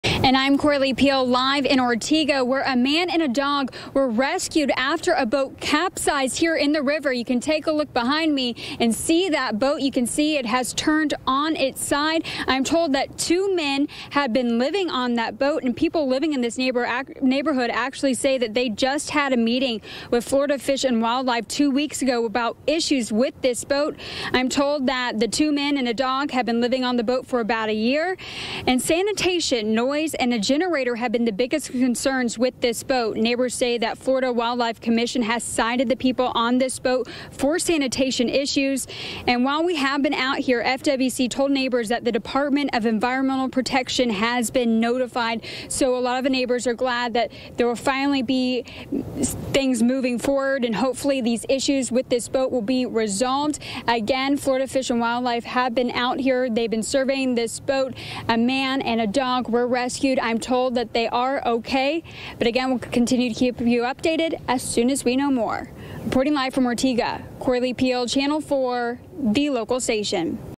Man and dog rescued from capsized boat
A man and his dog escaping a boat capsizing in the Ortega River, no one injured.